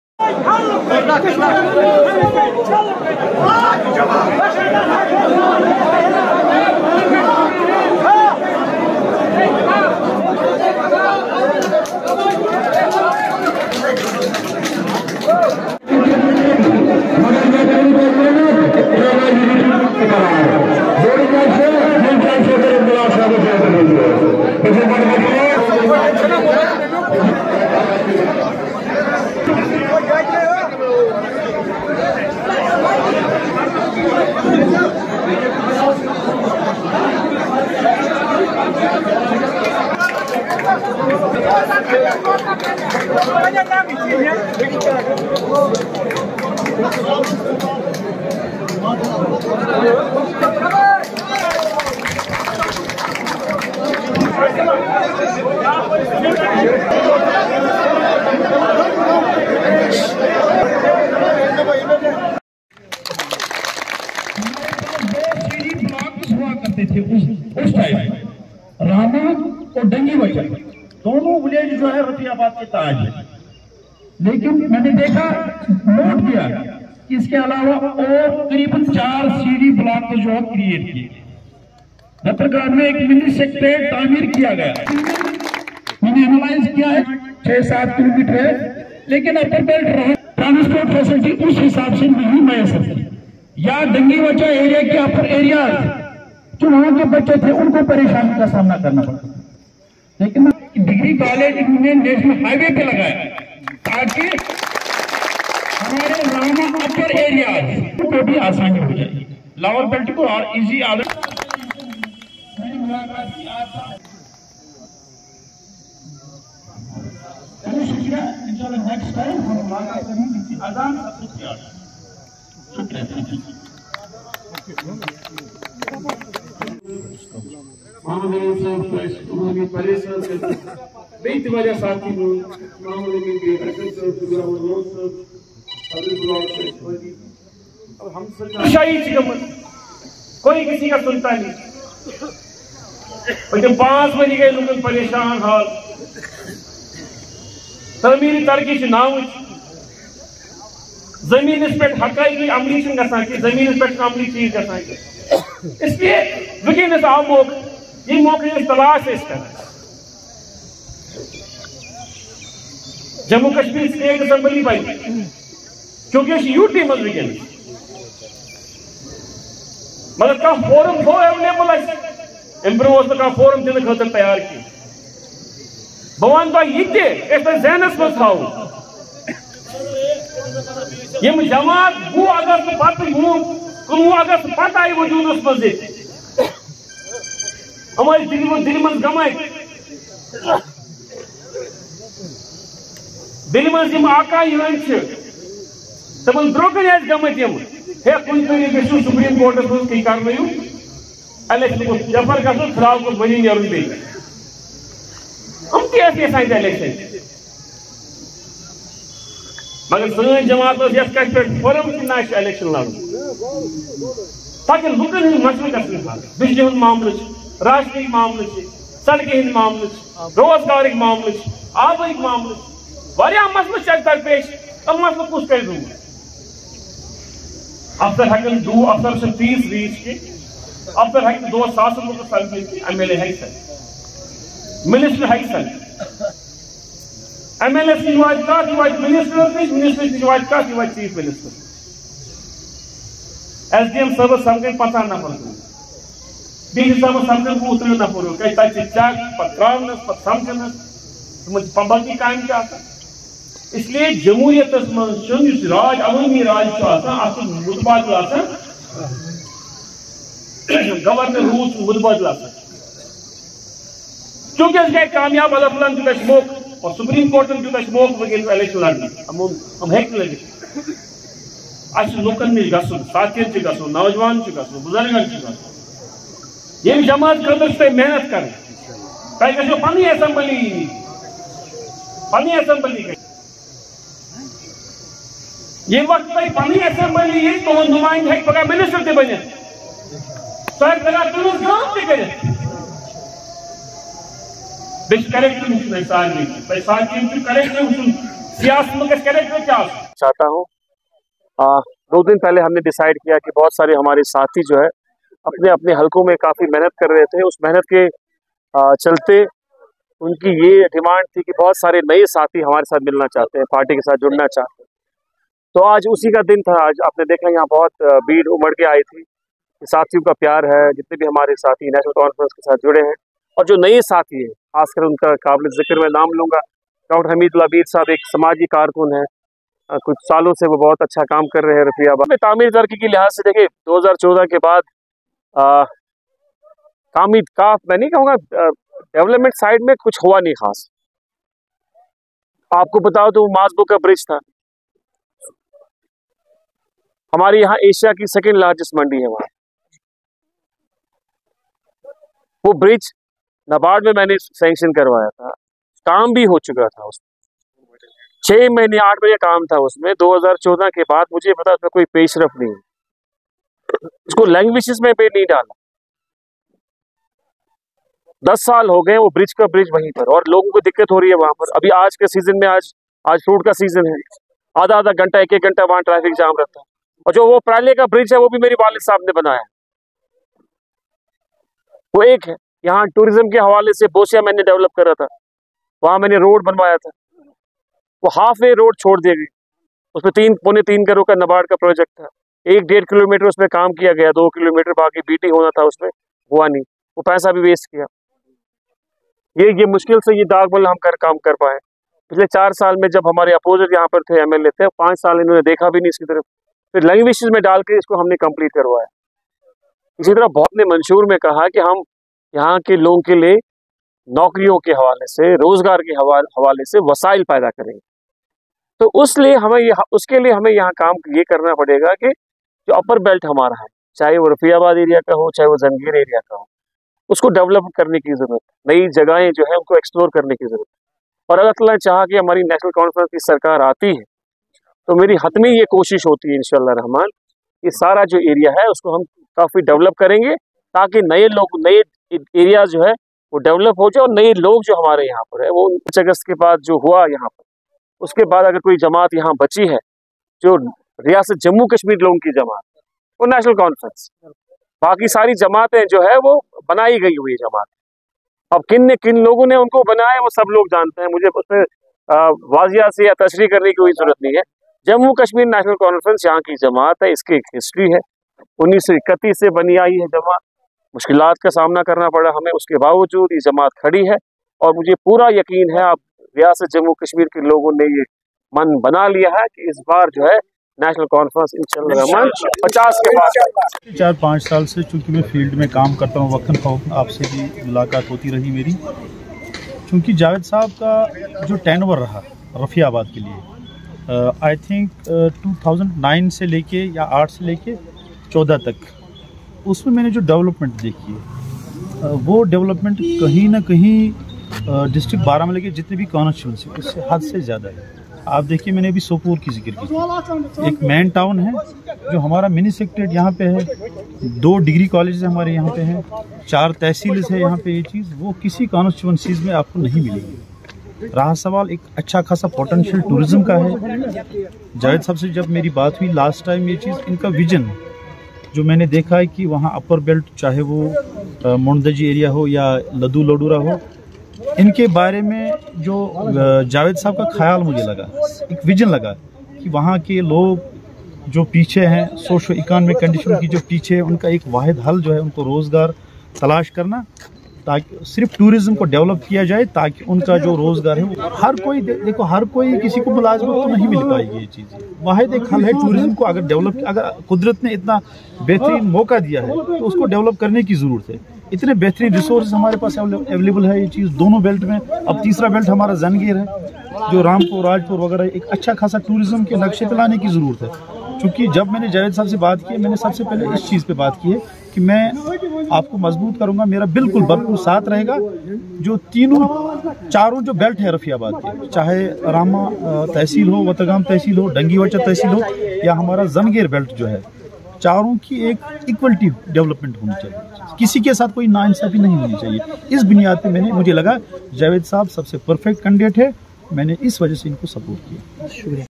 National Conference Workers meeting held at Dak Bungalow Chichama Rafiabad. Senior NC Leader
National Conference Workers meeting held at Dak Bungalow Chichama Rafiabad.

Senior NC Leader and Rafiabad Assembly Segment candidate Javaid Dar among party leaders and supporters were present in the meeting. Scores of People from Rafiabad and Adjoining areas joined the NC in presence of NC leader and assured the party leaders that full support will be given to NC in upcoming  elections.

Speaking to media NC leader Javed Dar said that